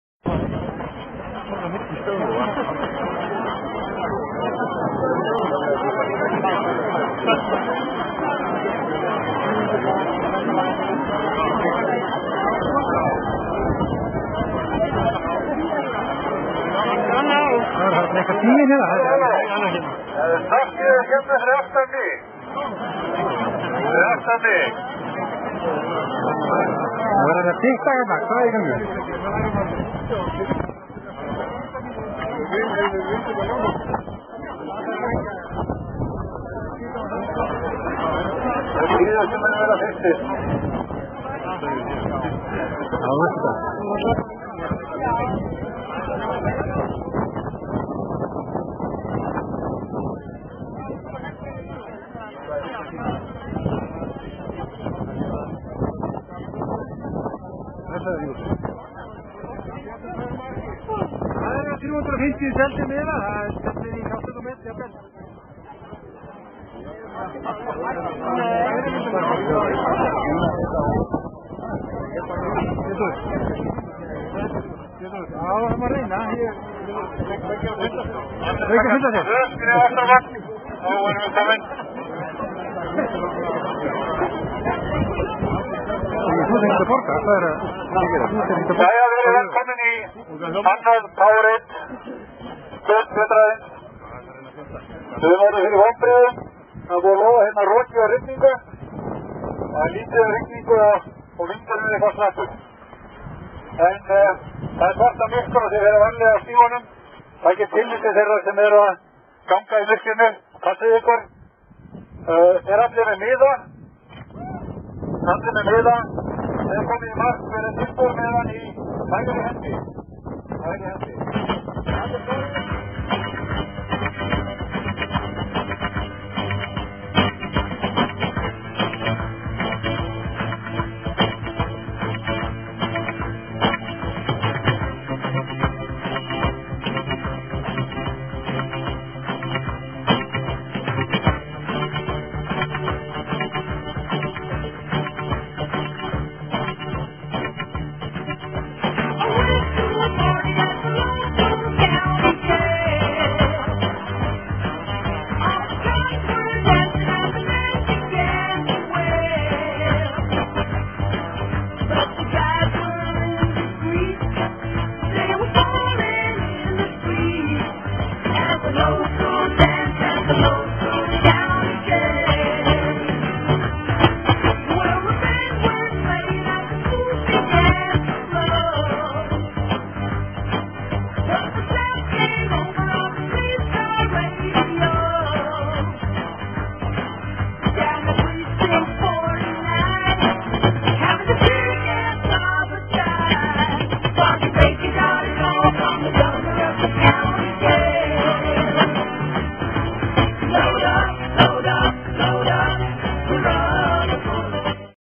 Powerade vetrarhlaup nr 2, 2012 2013
Startið á öðru Powerade vetrarhlaupinu 8. nóvember 2012